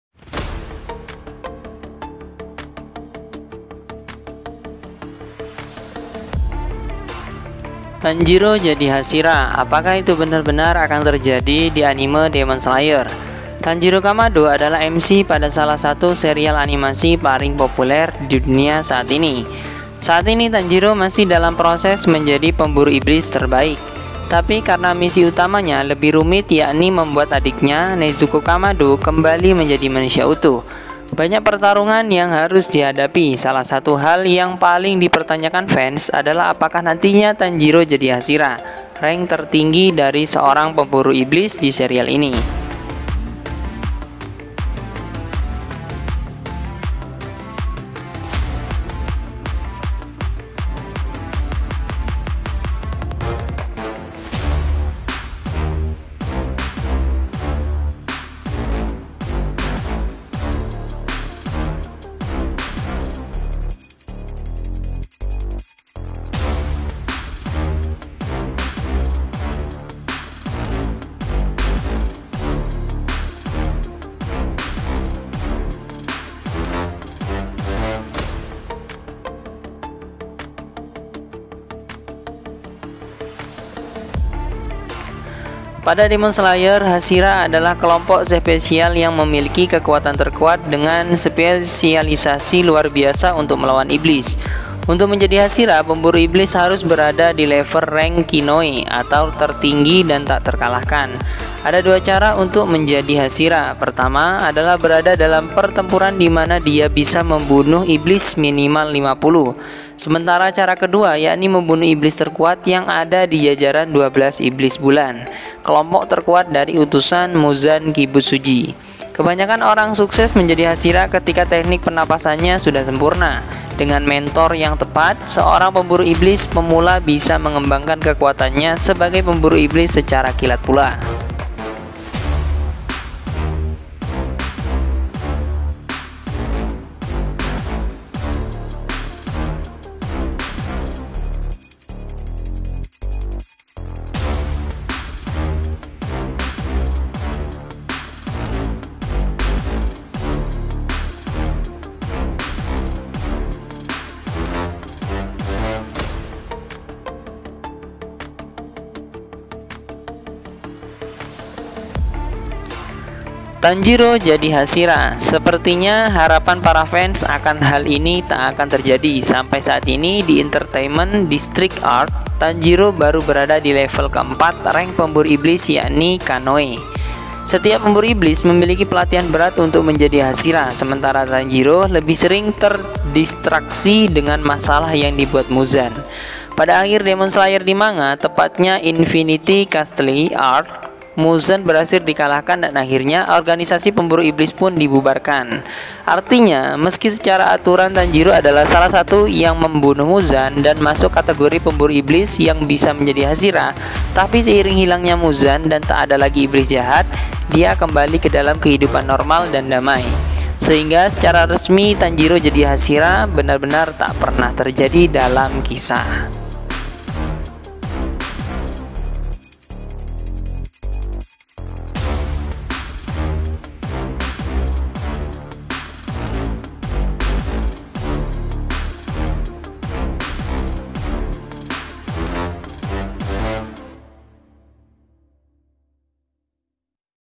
Bocoran ending Demon Slayer: Apakah Tanjiro jadi Hashira?
"Saksikan video ini untuk mendapatkan bocoran eksklusif tentang ending yang sangat dinantikan dari Demon Slayer! Kami akan membahas spekulasi seputar apakah Tanjiro Kamado, sang protagonis utama, akan menjadi seorang Hashira. Dengan perjalanan epik yang telah dilaluinya, pertanyaan besar muncul mengenai nasib Tanjiro dan apakah ia akan menjadi bagian dari elite pemburu iblis Hashira.

Kami akan mengulas petunjuk dan foreshadowing dari manga Demon Slayer, serta menyajikan teori-teori menarik yang telah muncul di komunitas penggemar. Apakah Tanjiro akan mengambil peran penting dalam Korps Pemburu Iblis, ataukah ia akan memilih jalur yang berbeda?

Jangan lewatkan kesempatan untuk mendapatkan wawasan mendalam tentang ending yang akan mengubah segalanya. Segera tonton video ini dan bergabunglah dengan diskusi kami untuk memecahkan misteri apakah Tanjiro Kamado akan menjadi seorang Hashira dalam akhir cerita Demon Slayer yang epik ini!"

★ COPYRIGHT / LICENSING ★